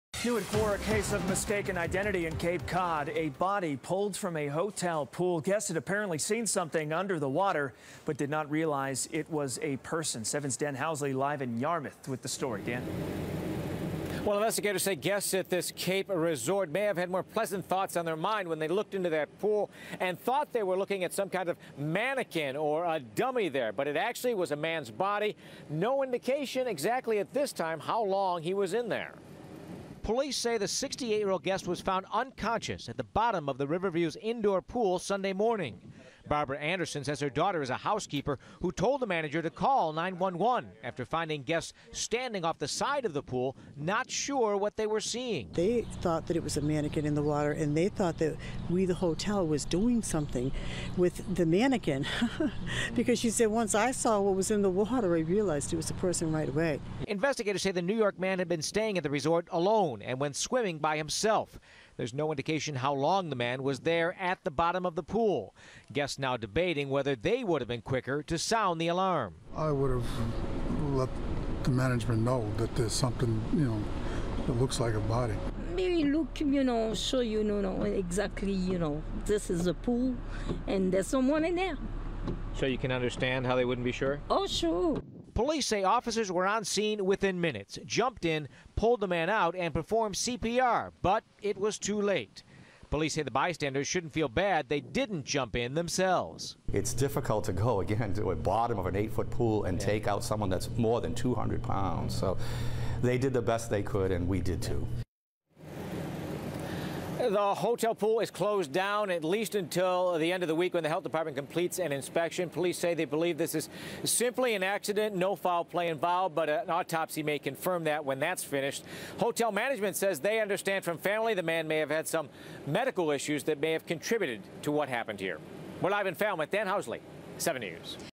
NY man drowned in Yarmouth indoor pool
YARMOUTH, Mass. (WHDH) - A body was pulled from a hotel pool in Yarmouth after guests saw it in the water, but didn't realize it was a person.

Police sid the 68-year-old guest was found unconscious at the bottom of the Riverview Resort's indoor pool Sunday morning.

Barbara Anderson said her daughter is a housekeeper at the hotel and told the manager to call 911 after finding guests standing off the side of the pool, unsure of what they were seeing. Many thought it was a mannequin or a dummy.

"They thought that it was a mannequin in the water and they thought we the hotel was doing something with the mannequin because she said once I saw what was in the water, I realized it was a person right away," Anderson said.

Investigators said the New York man had been staying at the resort alone and went swimming by himself. There's no indication how long the man was at the bottom of the pool.

Officers were on scene within minutes, jumped in and pulled the man out to perform CPR, but it was too late.

Guests are now debating whether they would have been quicker to sound the alarm.

"I would have let the management know that there's something, you know, that looks like a body," said guest Ron Pfister.

Police said the bystanders shouldn't feel bad they didn't jump in themselves.

"It's difficult to go, again, into the bottom of an 8 foot pool and take out someone that's more than 200 pounds. So they did the best they could and we did too," said Deputy Chief Steven Xiarhos of the Falmouth Police Department.
The hotel pool is closed down until at least the end of the week when the health department completes an inspection.

Police said they believe it was an accident and no foul play was involved. An autopsy will confirm the cause of death.

Hotel management said the family stated the man may have had medical issues that contributed to the incident.